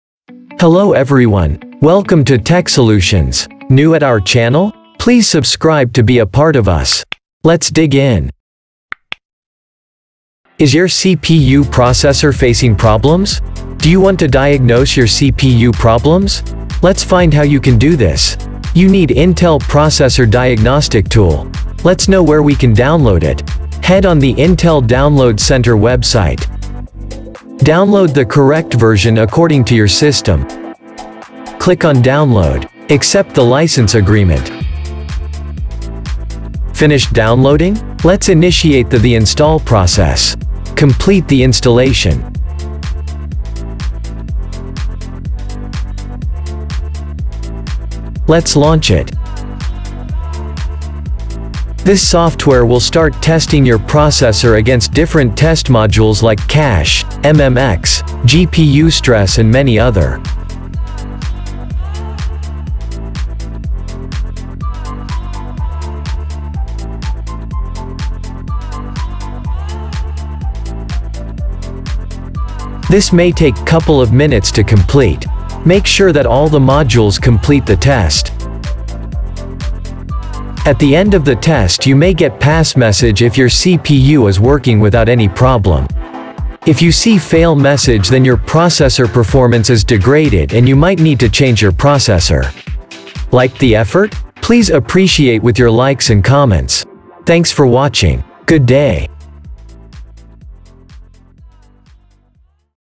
Download Intel Processor Diagnostic Tool | Processor Floating Point Test | Cache Size | MMX & SSE
The purpose of the Intel® Processor Diagnostic Tool is to verify the functionality of an Intel® microprocessor. The diagnostic tool checks for brand identification, verifies the processor operating frequency, tests specific processor features, and performs a stress test on the processor.

The Intel® Processor Diagnostic Tool release 4.1.5.37 is compatible with multiprocessor systems.
Intel Processor Diagnostic Tool is a simple-to-use program with a name that pretty much explains its functionality - it is capable of performing several benchmarks on your computer, in order to test the features of your Intel processor.
Upon initialization Intel Processor Diagnostic Tool automatically starts the scanning procedure. During this time it is highly recommended you stop your regular activity on the workstation, in order to prevent any unwanted errors and report glitches.
The resulted page shows plenty of information related to the Intel processor, such as the test start and end time, temperature test, detected brand string, CPU frequency, base clock, floating point test, cache size, MMX and SSE capabilities check, memory size and USB devices.